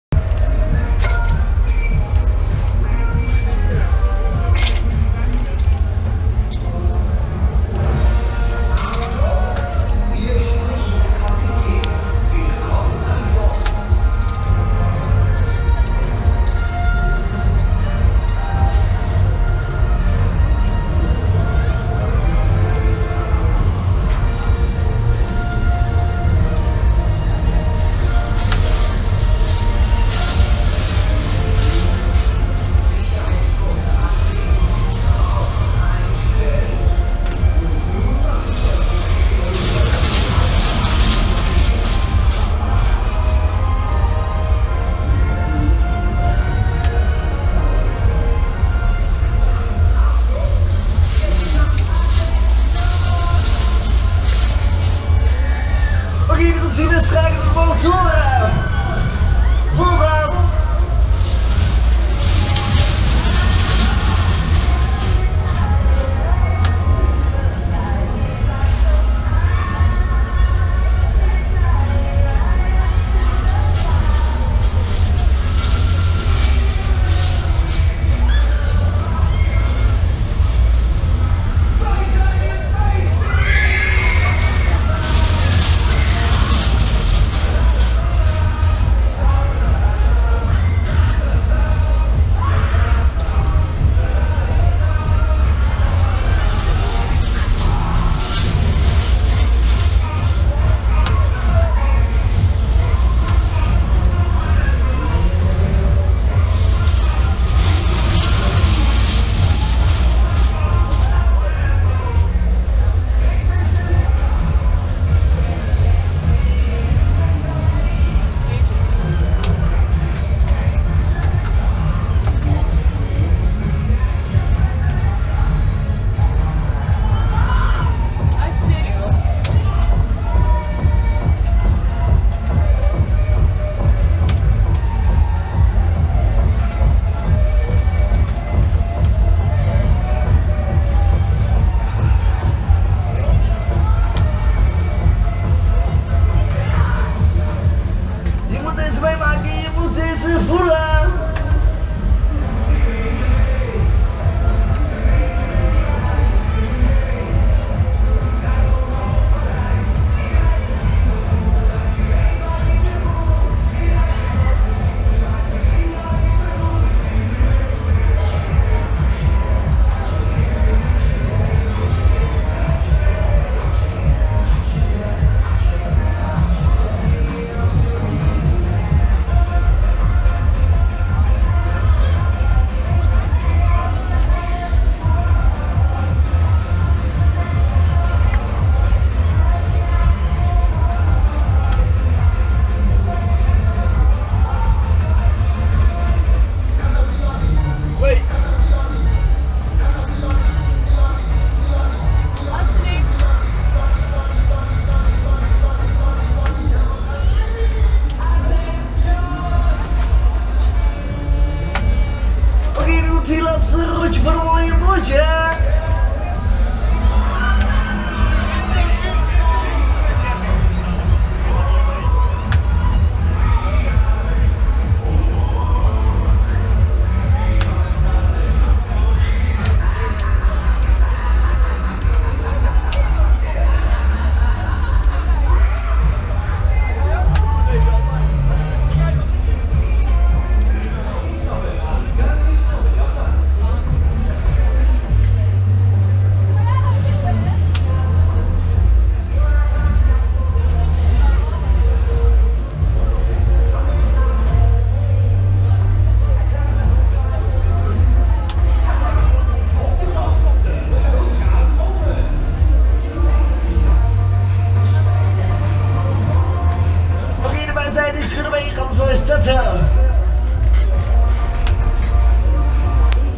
"EXPERIENCE" (VELTHUIS/MULDER) OPERATOR VIEW @ DE TILBURGSE KERMIS (THE NETHERLANDS) 2016
Manufacturer: KMG
Type: Experiënce
Owner: Velthuis/Mulder